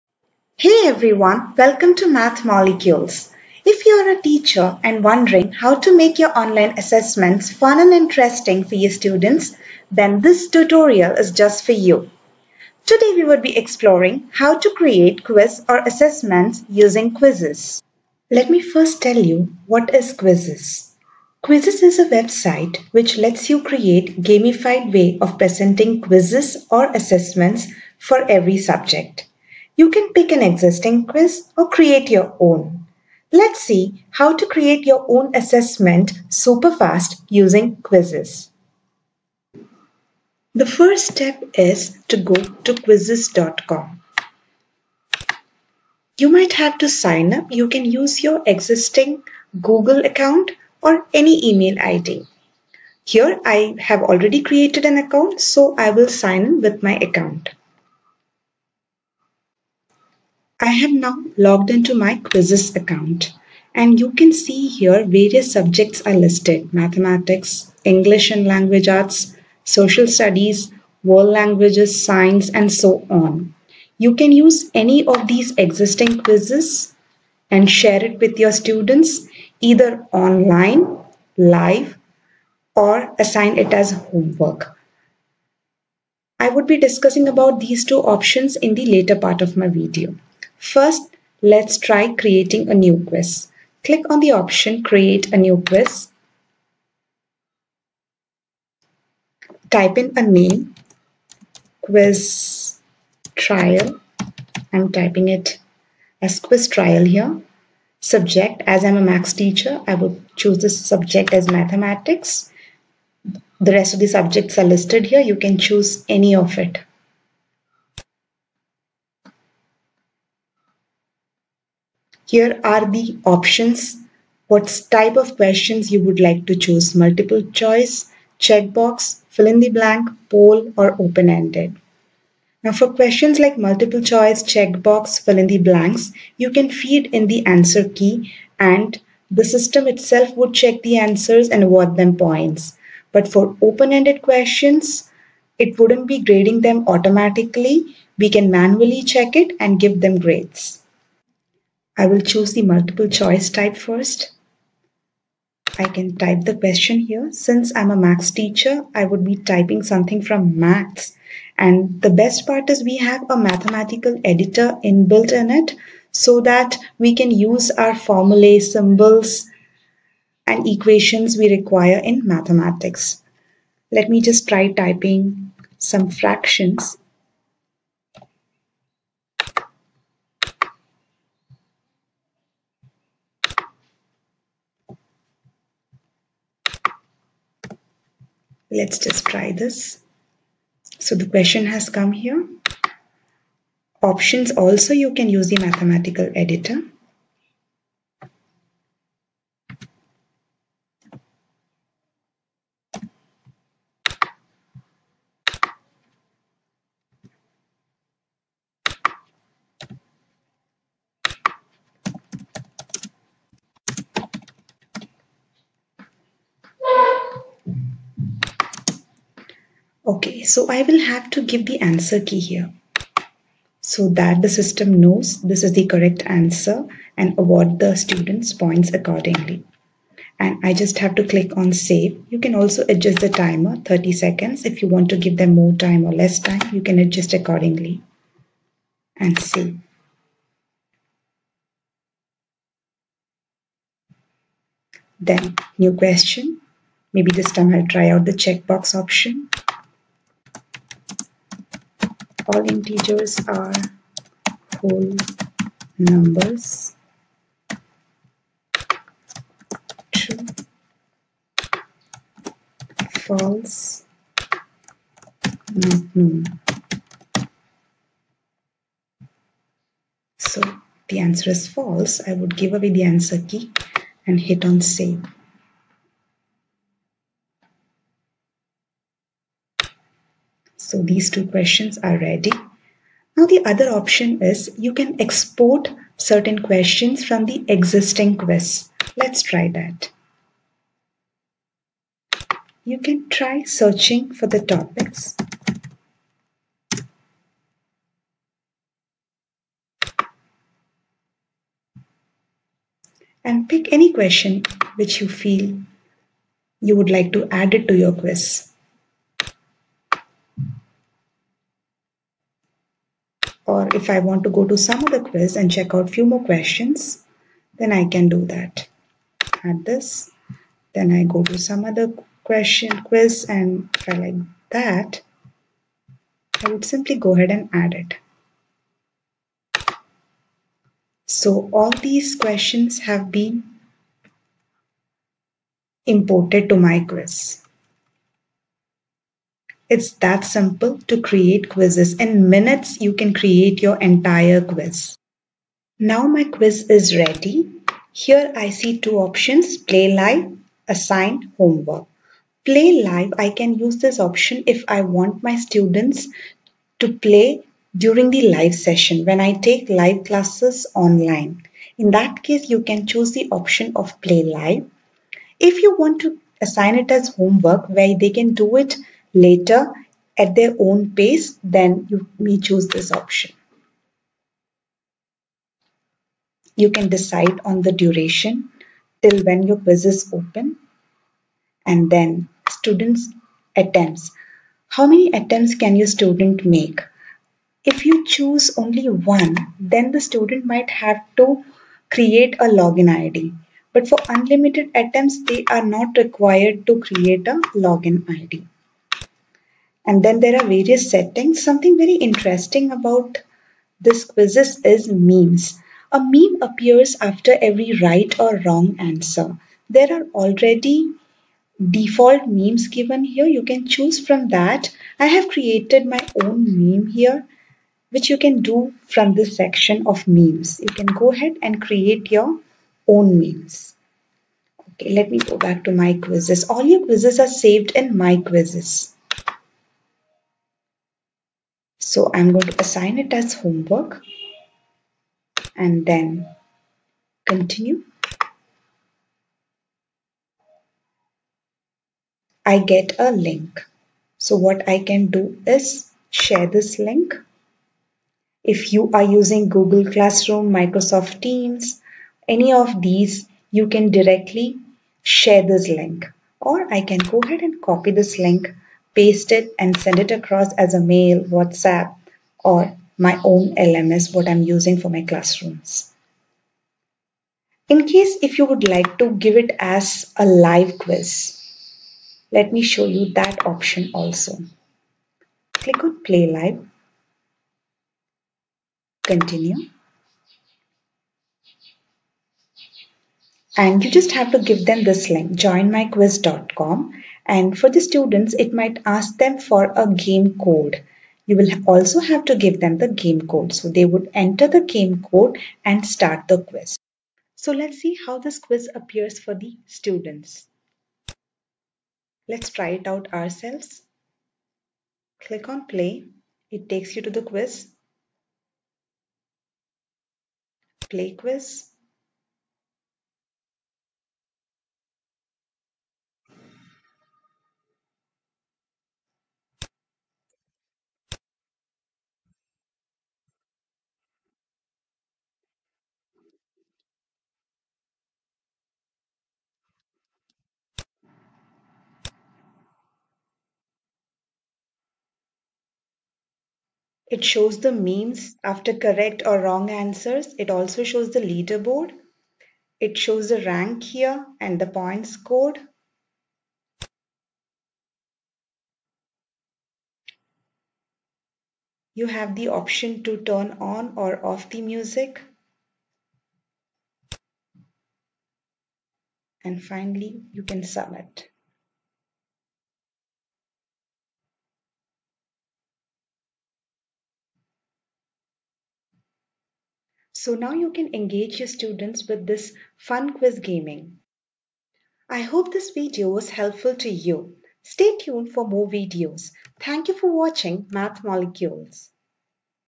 QUIZIZZ : GAMIFY your online assessments.Super quick and a fun way to engage students online.
Teachers can learn how to make online classes exciting and filled with fun by creating online tests / assessments / quizzes using QUIZIZZ. Tests turn into games . Get more involvement in classroom from your students. Use Quzizz to create games , check previous knowledge or reinforce a concept . Save time . Use ready made pre existing Quizzes or create your own with ease. Play live Quizzes or assign as home assignment.